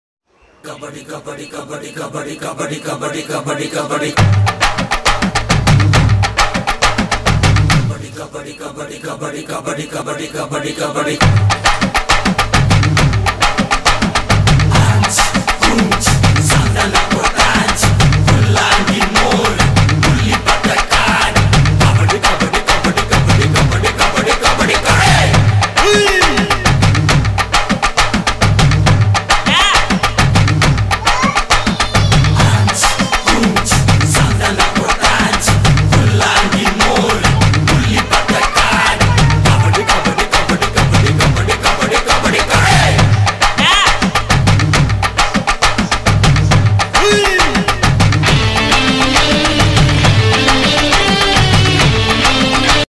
Dog and Cat playing Funny Kabbadi match
Dog and cat playing kabbadi match and watch their cute fight previous video of our channel